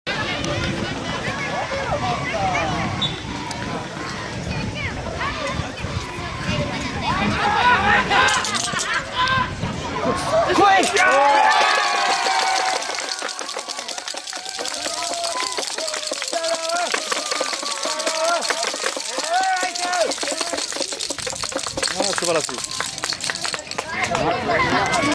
札幌Ｕ－12＠全日本少年大会
2009全日本少年サッカー大会グループＧ。札幌Ｕ－12vs.ばらきSC。後半４分。北川のダイビングヘッドのゴール。